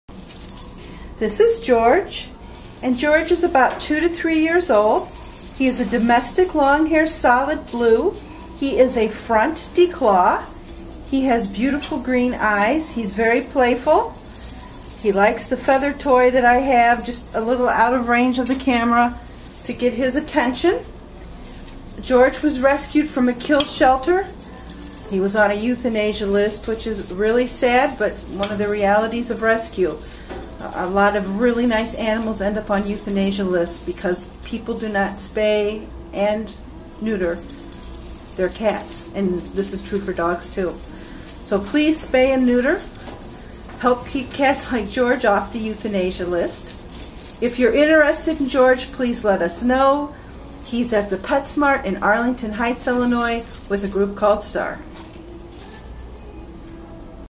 George
George is a 2-3 year old front declaw cat  looking for a home.